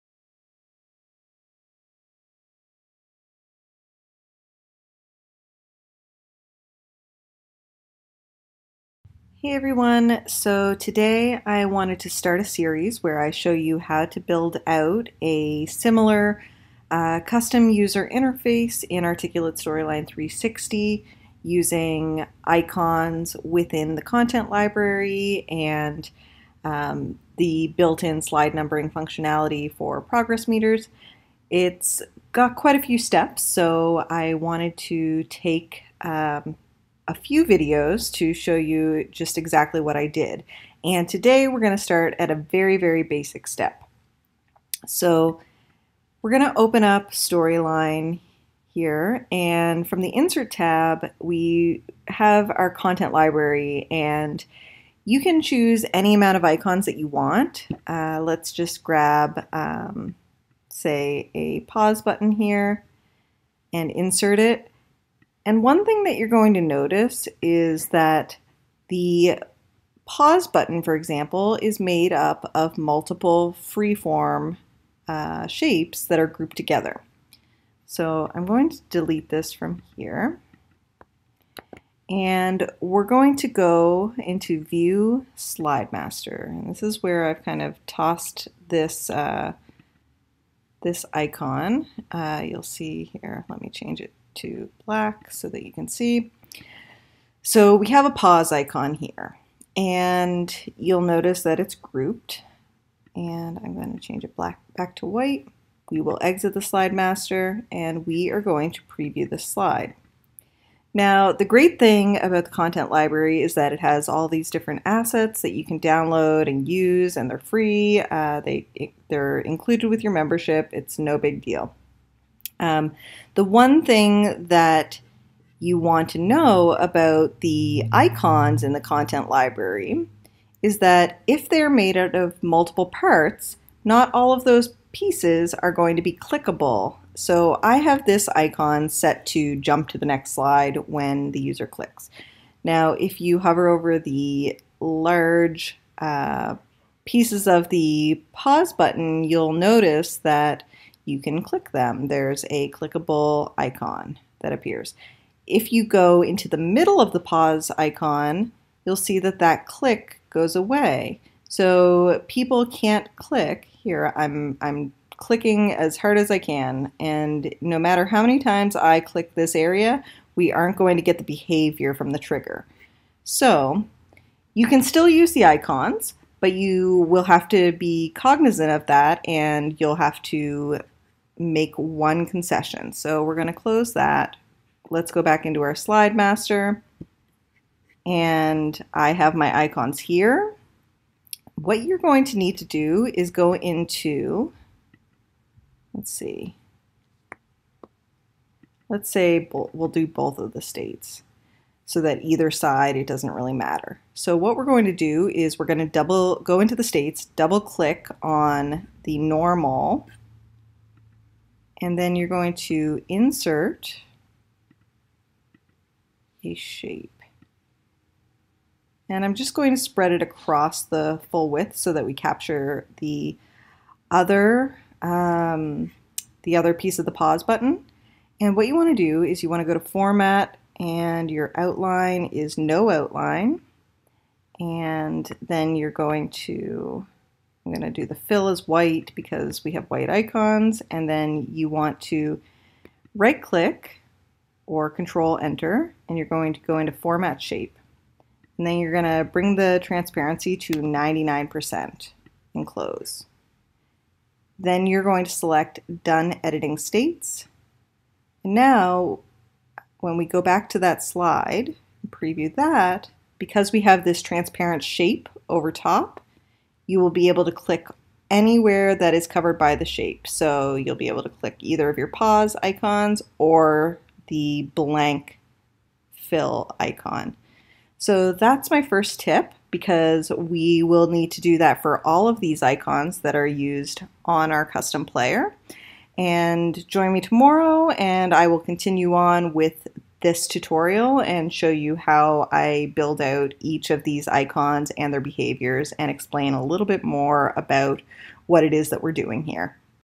Creating a Custom User Interface in Articulate Storyline 360 - Part 1
In this screencast, I'm sharing one of the basic fundamental tips for working with icons from the content library in Articulate Storyline 360. This tip will be critical when it comes to using the content library icons to build out your custom navigation in the coming screencasts.